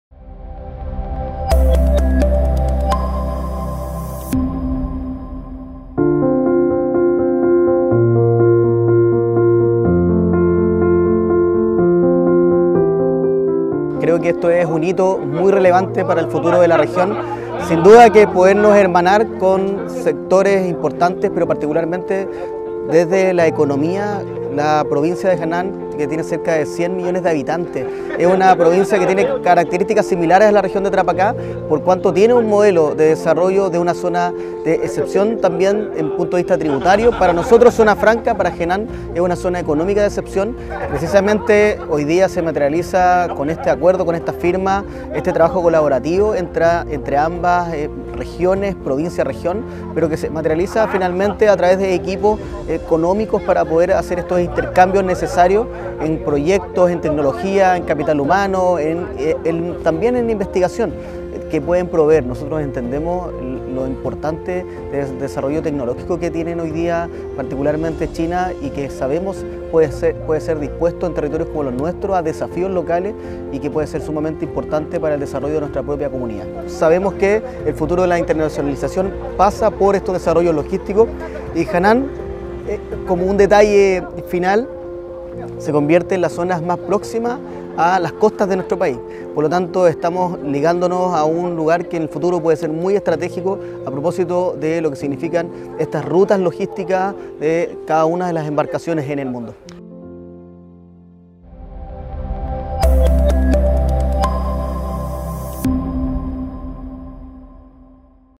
Tarapacá y Hainan firmaron una carta de intenciones de amistad.
Gobierno Regional de Tarapacá - Internacionalización 🇨🇱🇨🇳

En Iquique, el Gobernador José Miguel Carvajal y el vicegobernador de la provincia de Hainan, en China, Shen Danyang, firmaron una carta de intenciones de amistad entre el Gobierno de Tarapacá y esta región considerada la "Hawaii de China", documento que sentará las bases para que, en el futuro próximo, se firme un Acuerdo de Colaboración.

Conoce más detalles en esta nota. 👀📱